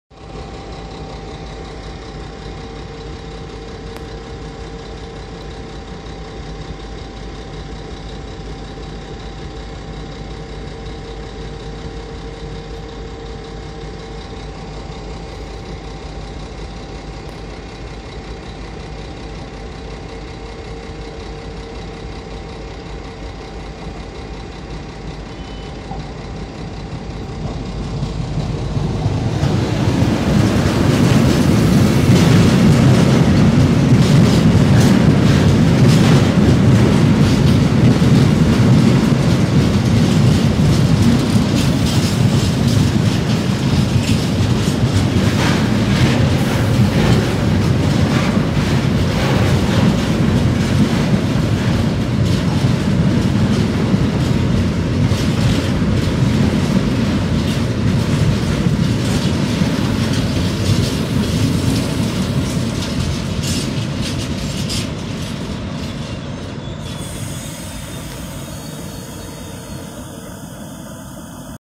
Cargo Eso, elektrická lokomotiva řady 363 (FHD)
Kotlová ucelenka s benzínem příjždí do rozestavěné stanice Křižanov, srpen 2020.